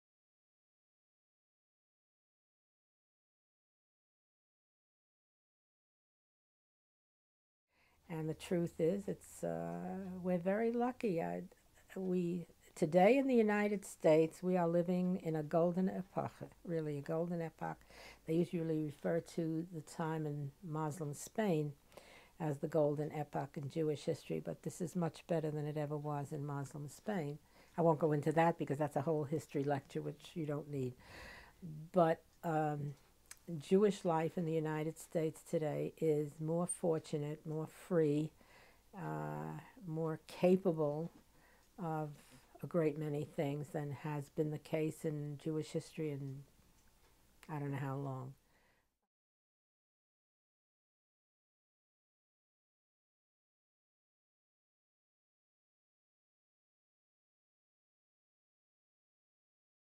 The Golden Epoch For Jews
Chana Schachner argues that right now in America is the golden age for Jews, rather than the time period in Muslim Spain.